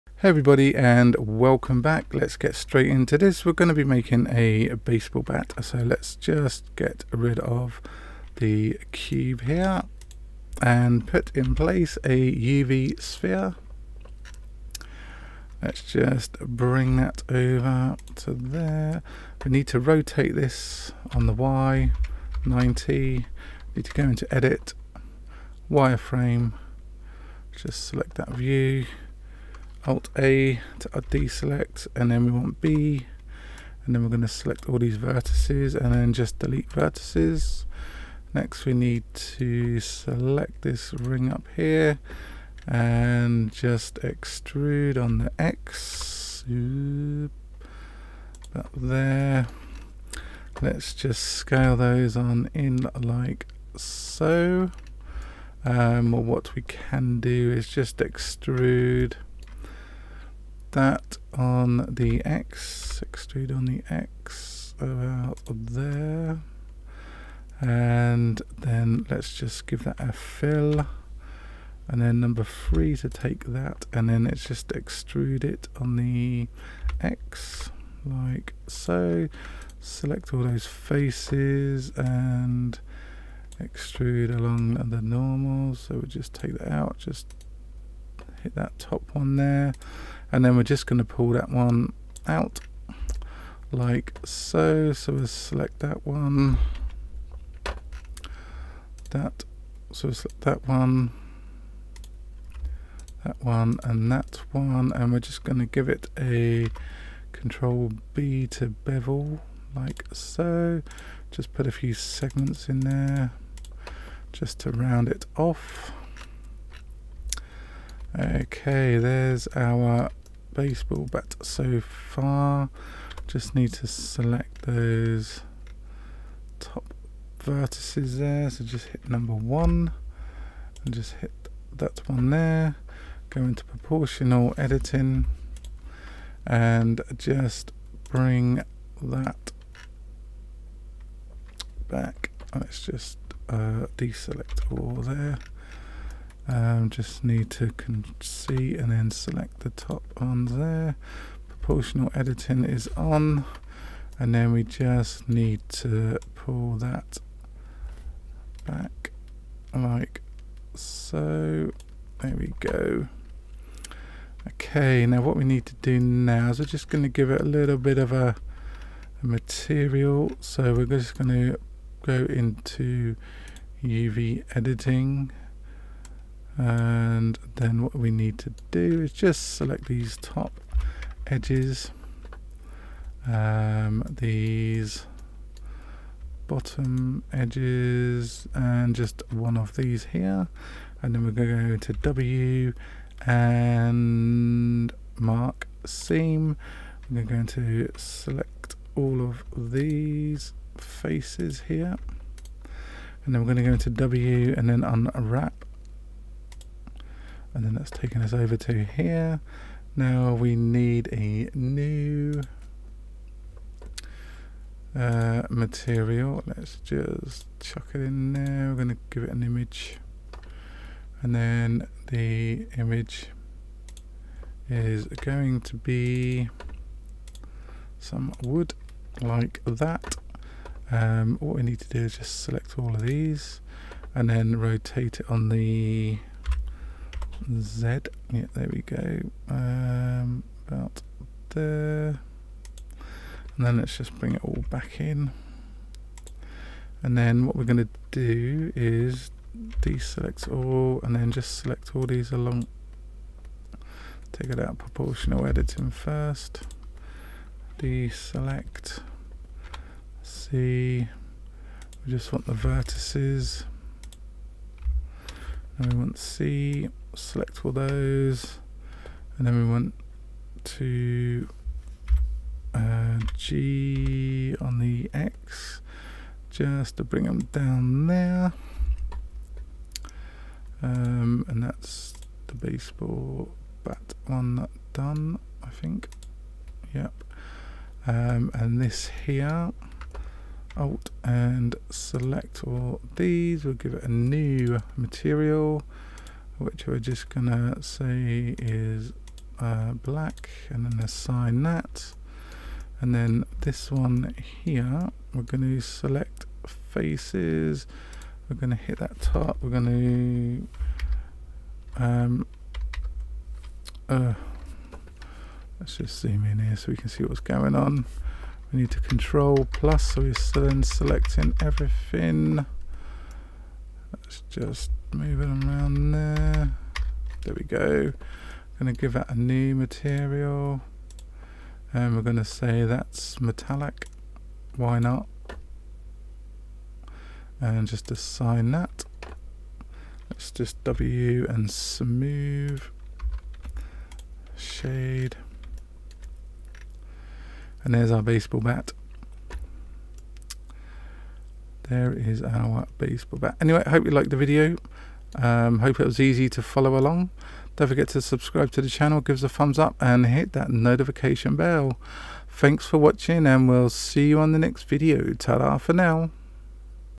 Blender 2.83 Tutorial: Modelling a baseball bat and material
Today I show you how to make a baseball bat in blender 2.83 and add wooden material to the base.
Easy Blender 3D tutorial to follow along with.

If there's something you want a tutorial on just let us know.